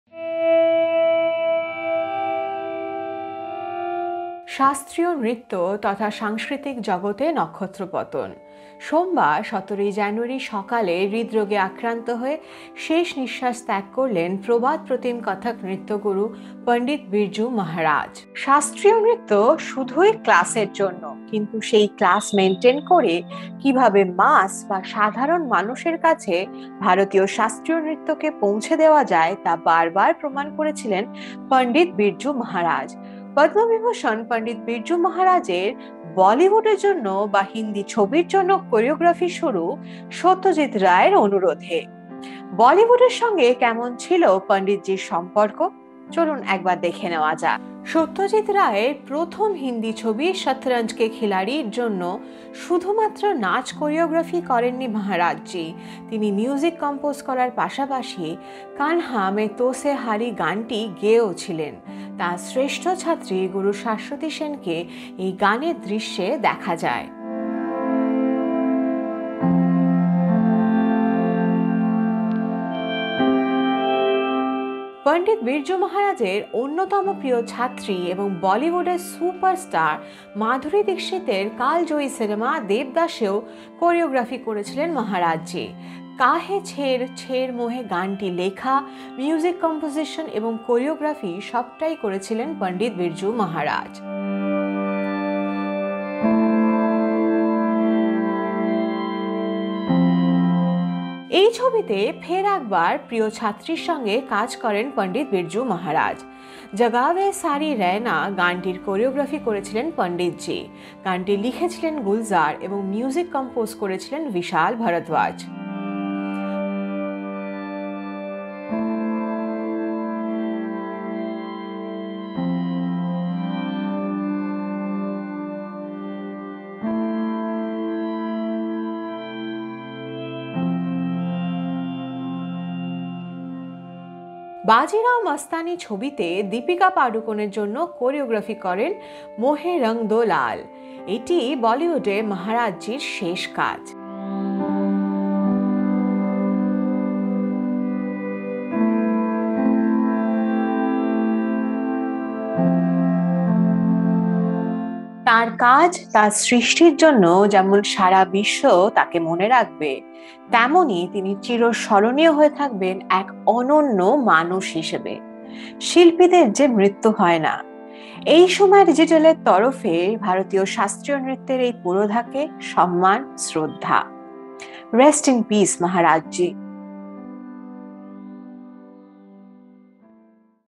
বলিউডকে সমৃদ্ধ করেছিলেন পণ্ডিত বিরজু মহারাজ| Pandit Birju Maharaj and his Bollywood Choreography
শাস্ত্রীয় নৃত্য শুধুই ক্লাসের। এই কথাই হয়তো বার বার শুনতে হয়, কিন্তু সেই ক্লাস মেন্টেন করে কীভাবে মাস বা সাধারণ মানুষের কাছে ভারতীয় শাস্ত্রীয় নৃত্যকে পৌঁছে দেওয়া যায়, তা প্রমাণ করে দিয়েছিলেন পদ্মবিভূষণ পণ্ডিত বিরজু মহারাজ। কিংবদন্তী পরিচালক সত্যজিত্‌ রায়ের অনুরোধে প্রথমবার হিন্দি ছবির জন্য নাচের কোরিওগ্রাফি শুরু করেন পণ্ডিতজী। কেমন ছিল বলিউডে সঙ্গে মহারাজজীর সম্পর্ক?